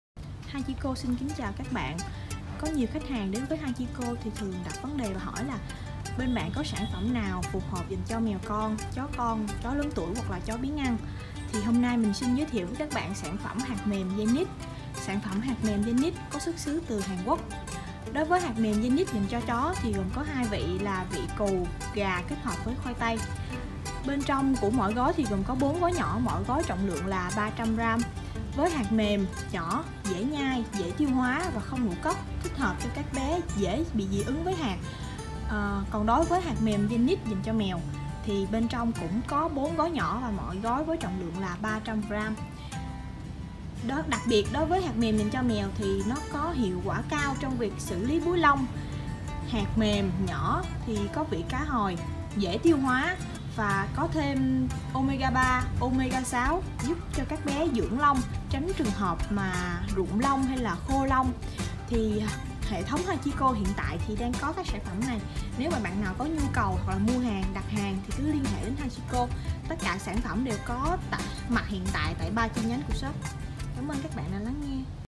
Thức ăn cho mèo con, chó con, mèo già chó già kén ăn
Hạt nhỏ mềm xuất xứ từ Hàn Quốc vừa mềm vừa thơm ngon.